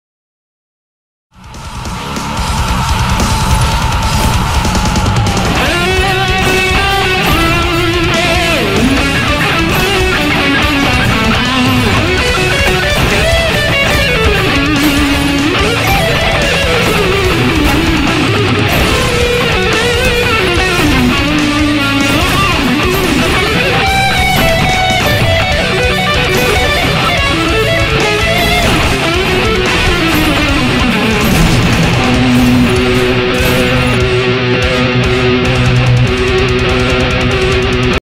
30 Headshots in 30 Seconds (Icebreaker)
As promised the second in the 30 in 30 playlist. Icebreaker. This majestic piece of weaponry was able to decapitate 30 unlucky guardians . The next gun wont do that. The next will incinerate them in arc light. That's right the Queenbreaker Bow is the next in line to have a 30 in 30 video. (That or Patience and Time.)